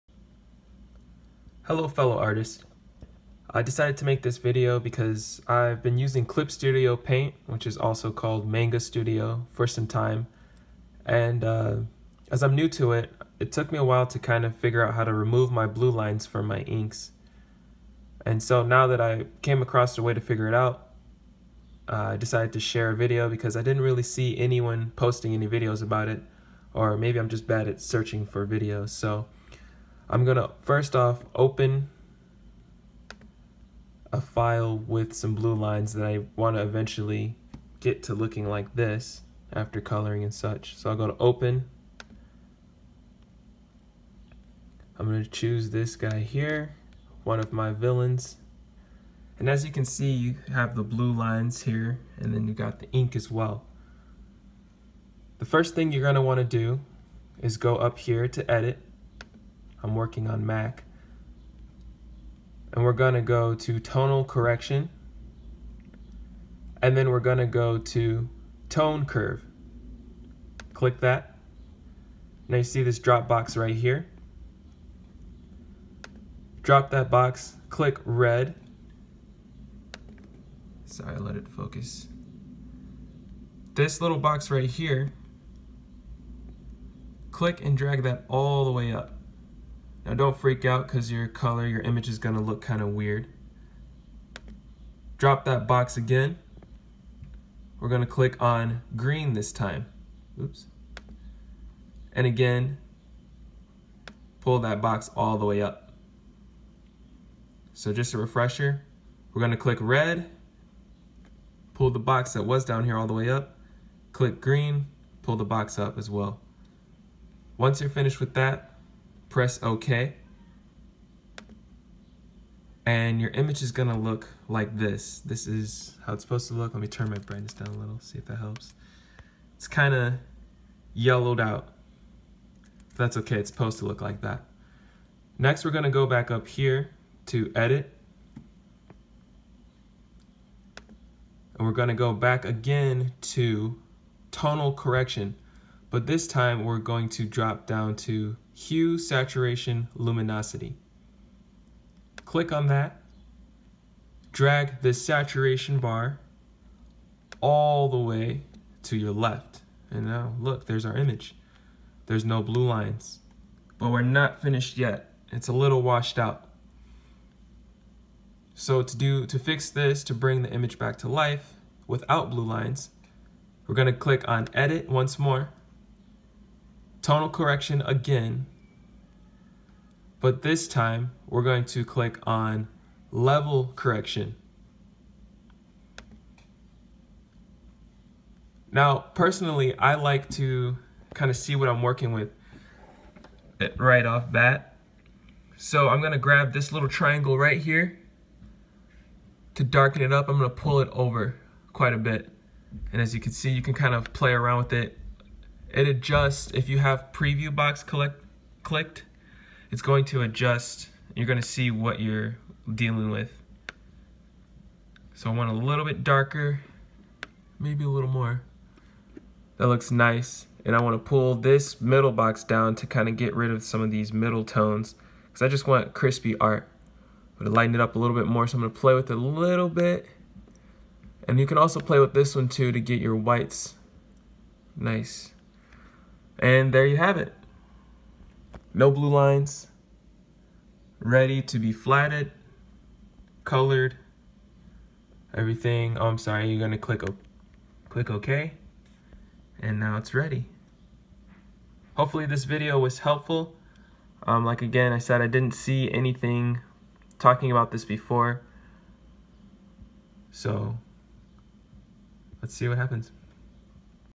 Remove blue pencil lines in Clip Studio Paint
This is a tutorial on how to remove blue pencil lines from an inked drawing so that you can begin to color your work on Clip Studio Paint.

I hope that this video is useful to artists who are learning and want to take the steps toward making their work look as professional as possible. Good luck!